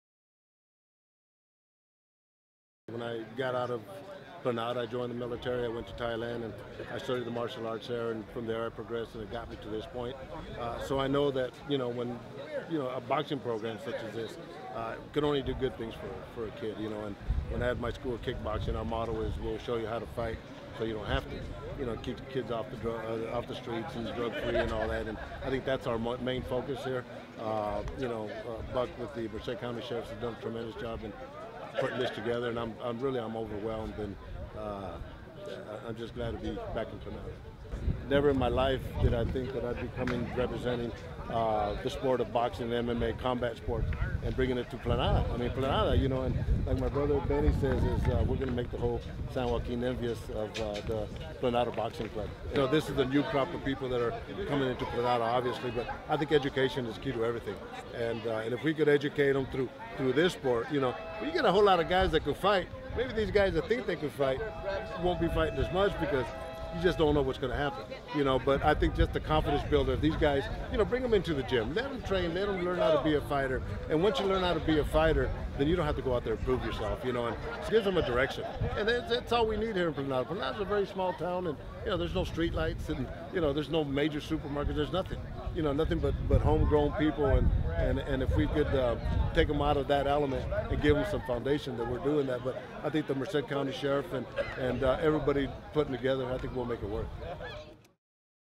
Jacob "Stitch" Duran and Merced County Sheriff bring boxing to Planada
Planada native and professional cutman Jacob "Stitch" Duran and the Merced County Sheriff's Department are working together to revive youth boxing in Planada. Duran was present for the Benefit for Boxing event at Cesar E. Chavez Middle School on Friday, March 20, 2015. (Andrew Kuhn/ .)